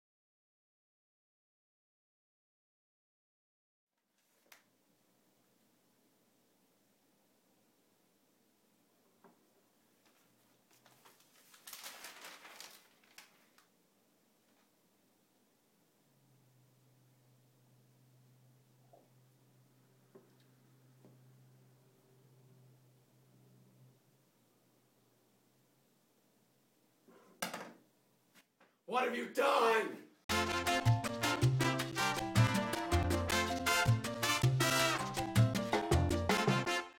The Man Who Overreacts to Everything
A comedy short I made.

Tags (ignore) - comedy short, sean capdeville, guy who overreacts too everything, vulcans forge films, my roommate is satan